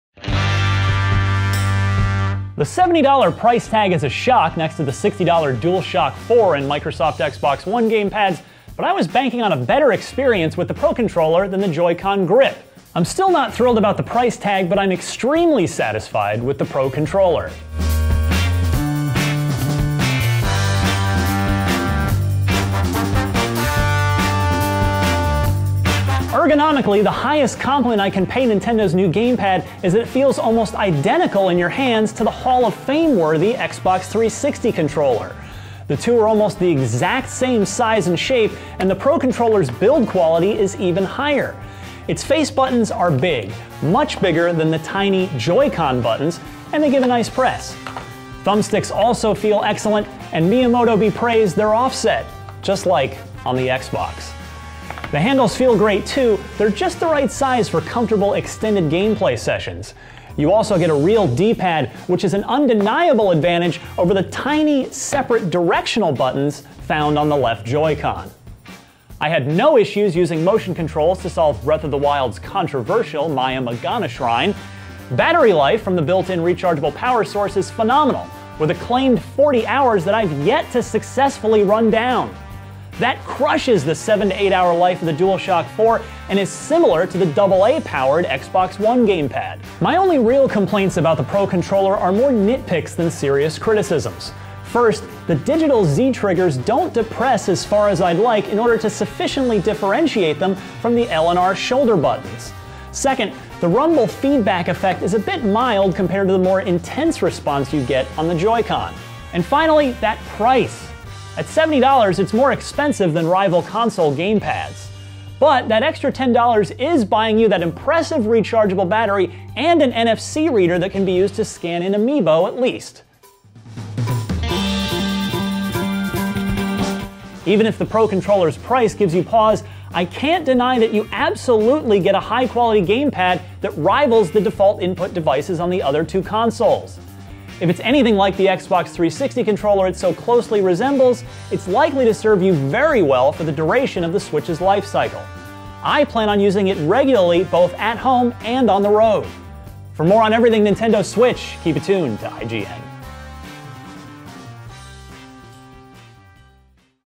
Nintendo Switch Pro Controller Review
Switch's Pro Controller costs a bit more than comparable gamepads, but it's a stellar gamepad with great battery life.

Follow IGN for more!